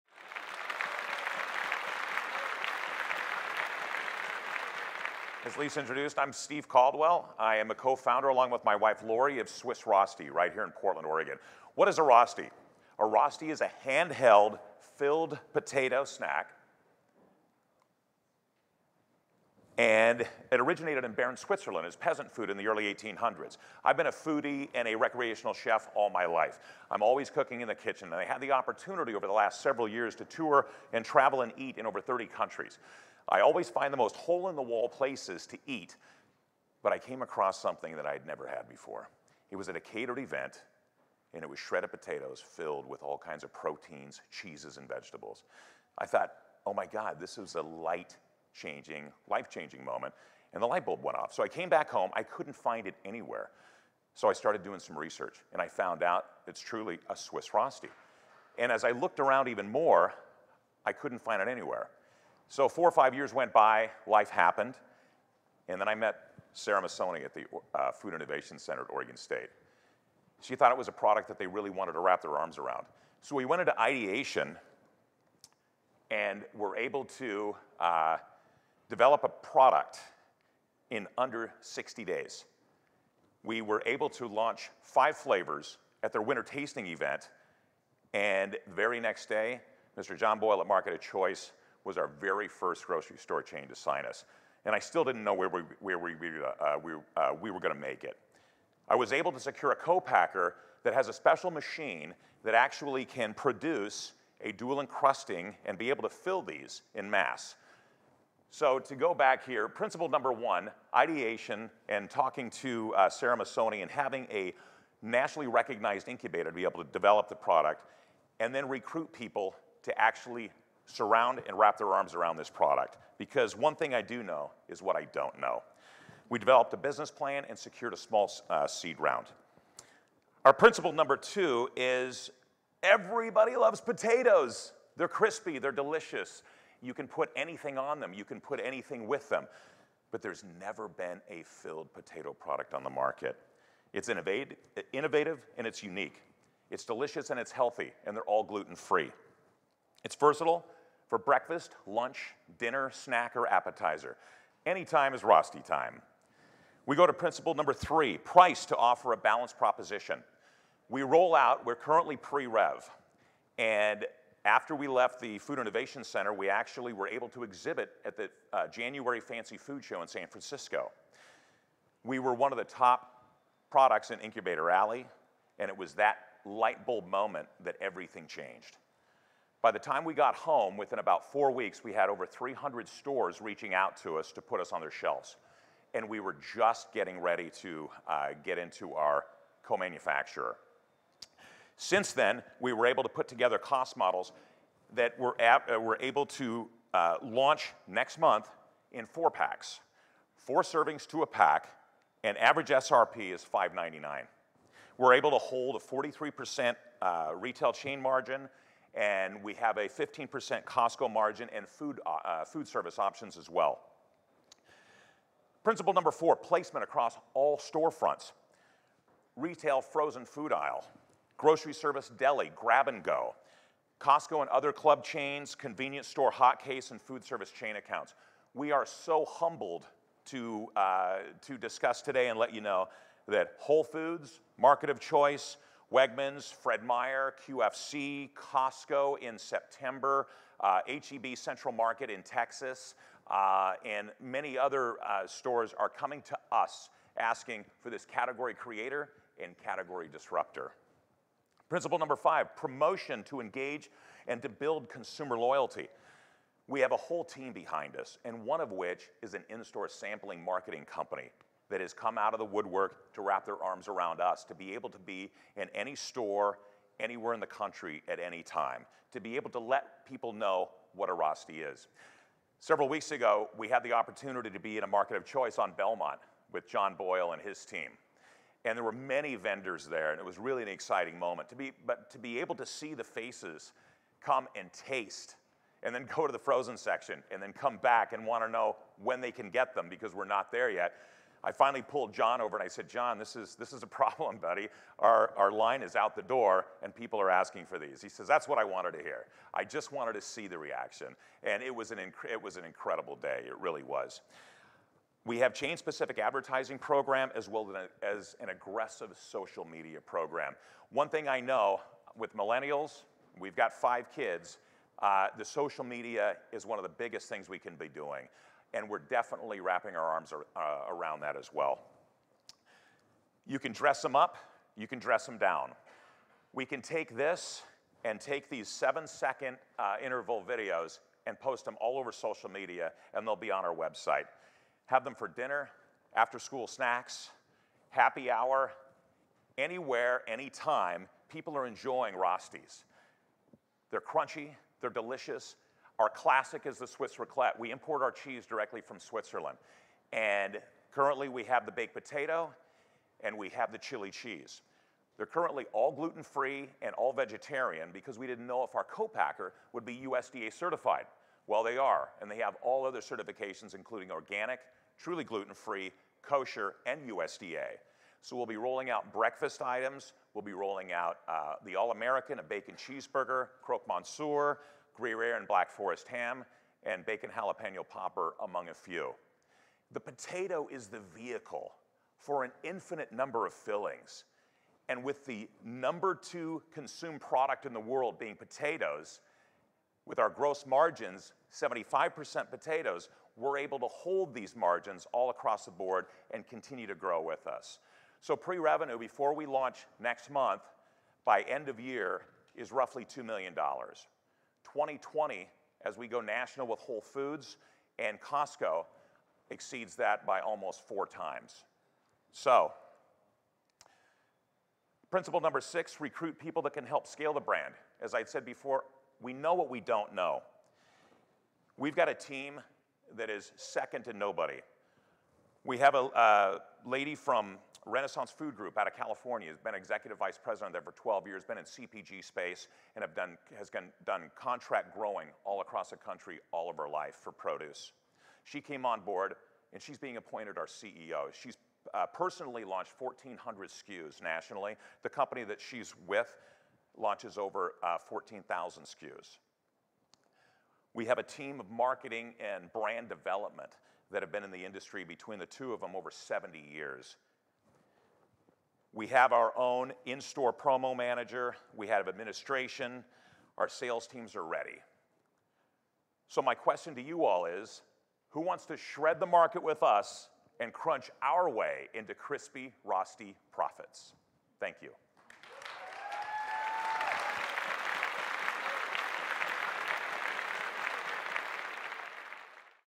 Stephen Caldwell of Swiss Rosti presents at the Angel Food Conference 2019
Stephen Caldwell of Swiss Rosti presents at the Angel Food Conference at The Redd East on June 19, 2019.

The Angel Food Conference & Education Series is a one-day angel conference featuring a panel discussion, pitch competition, and food fair. The OEN Angel Food Fund represents the state’s first fund designed exclusively for Oregon’s valuable food and beverage sector.

Videography: One Hundred Seconds
Video Editor: Miles Shattuck